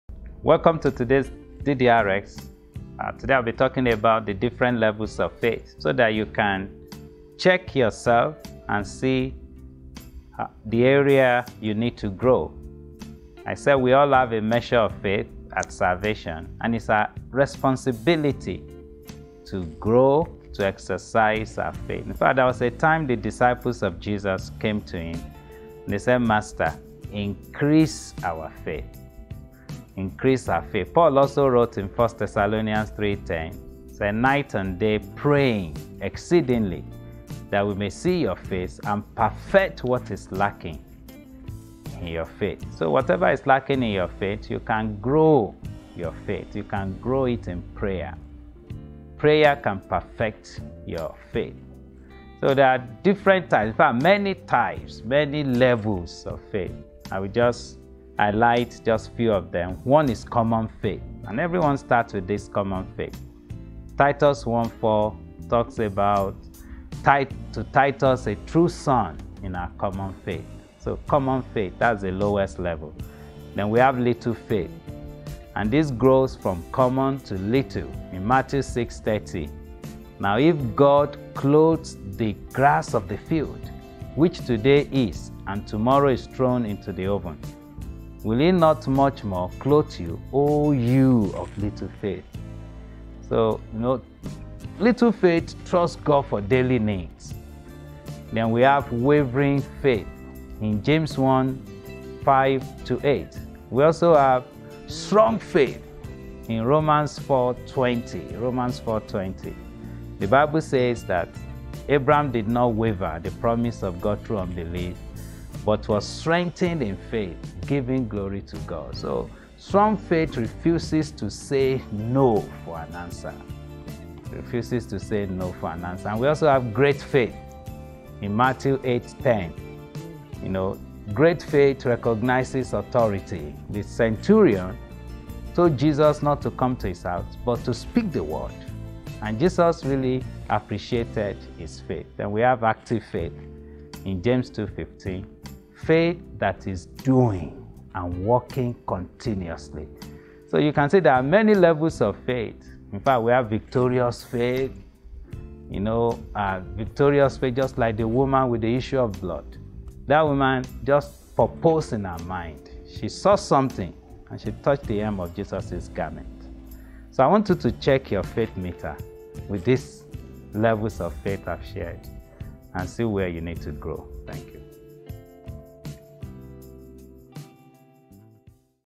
DDRxS14E03: Different Levels of Faith
In today's DDRx, Pastor Olayinka Dada explains to us the different levels of faith

Stay in Touch with Us!